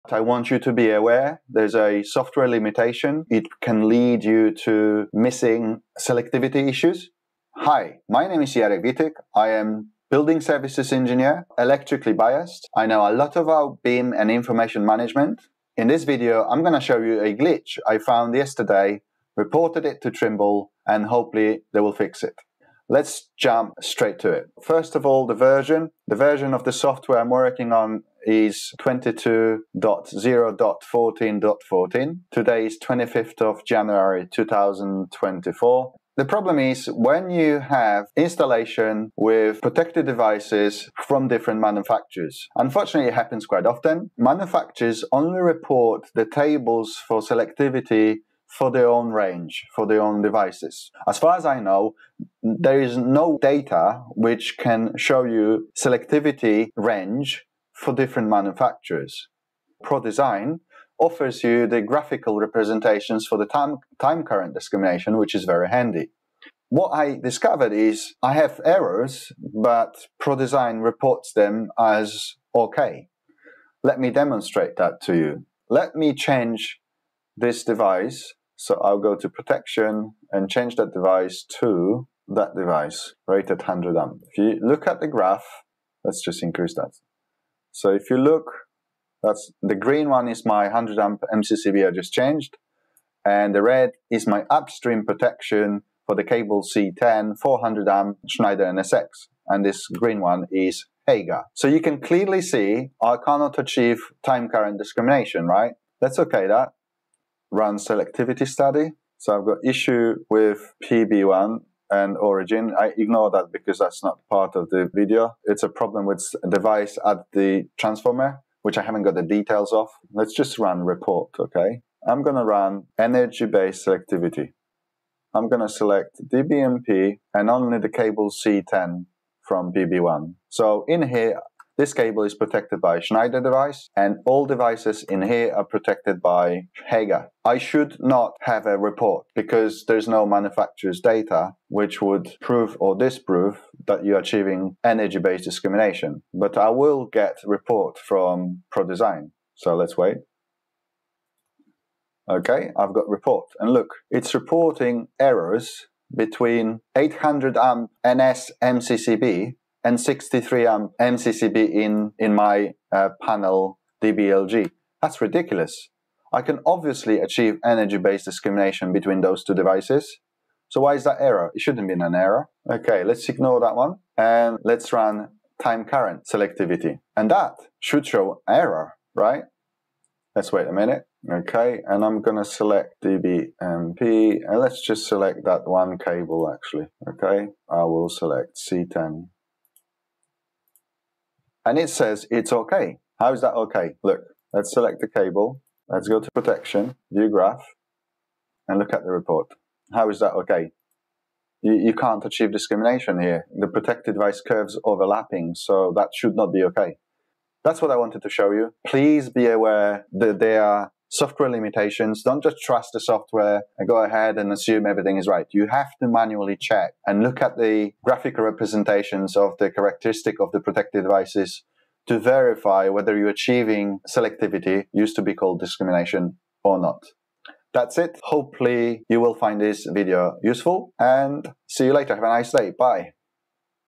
PD-05-01-01_Glitch_Selectivity
Description: Welcome to next episode in our electrical design series! In this video, I highlight glitch I encountered in Trimble ProDesign. This glitch involves incorrect reporting in Energy-Based and Time-Current selectivity studies, when your installation includes protective devices from various manufacturers.
I provide a detailed overview of the issue, demonstrating how ProDesign may not accurately reflect selectivity. This insight is vital as it impacts the reliability and safety of electrical installations.
Join me as I navigate through the software, highlight the glitch, and discuss its potential implications for your projects.